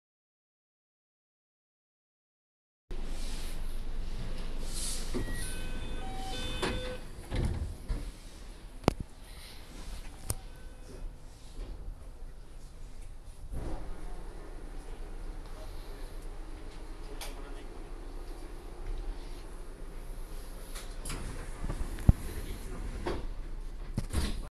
227系S03の半自動ドア開閉
あまり撮ることがなかったのですが撮れる日が来たので少しやってみました
撮影場所  可部駅